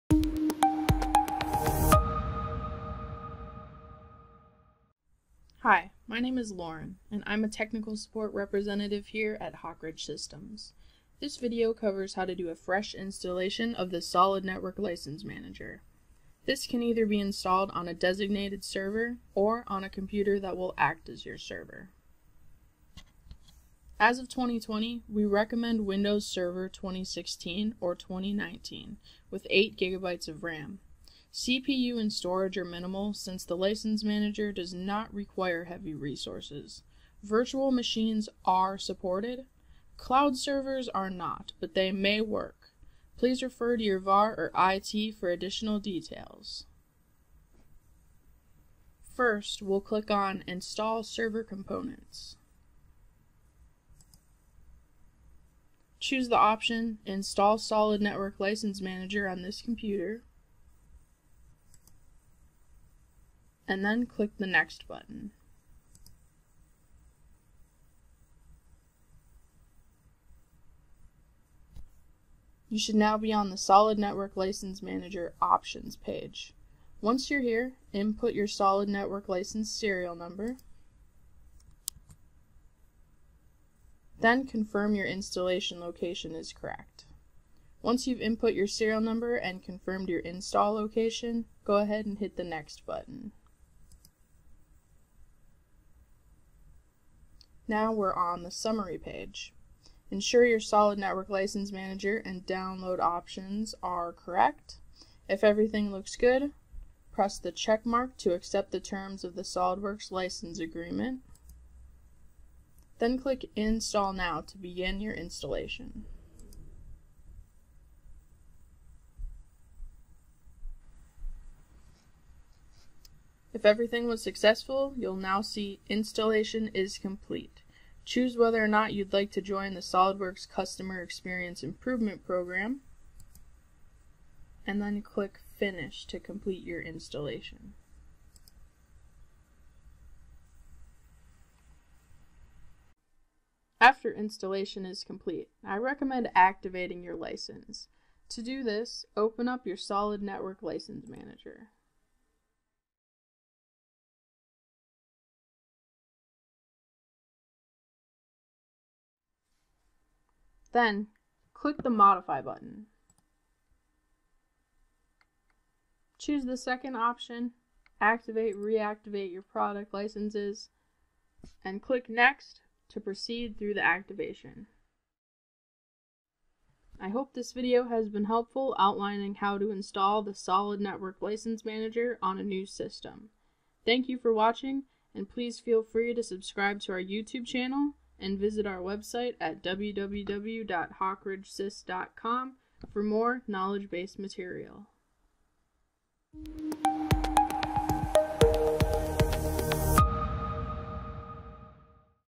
SOLIDWORKS Network License Manager Installation
Learn how to install the SolidNetWork License Manager (SNL) Server in this video. The SolidNetworkLicense Manager manages license handling of floating network licenses.